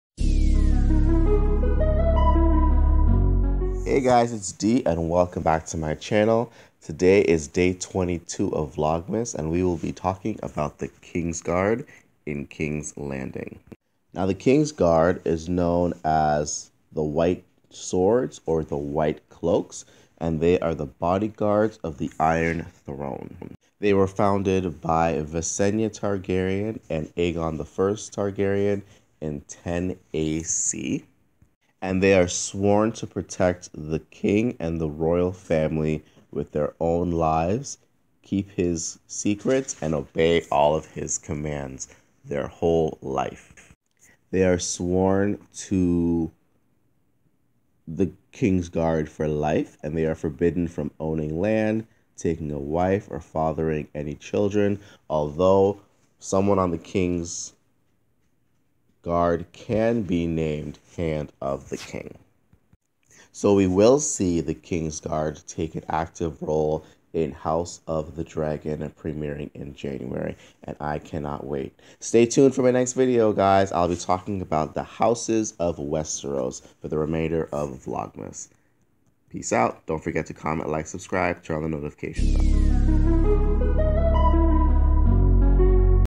THE KINGSGUARD ( VLOGMAS DAY 22)
It's VLOGMAS 2021
 I'm so so excited to be doing Vlogmas this year this is my first time, I'll be covering the history of House Targaryen and the charaters involved in the Dance of the Dragons, a civil war within House Targaryen

Preimers 2022

Peace Out ✌🏾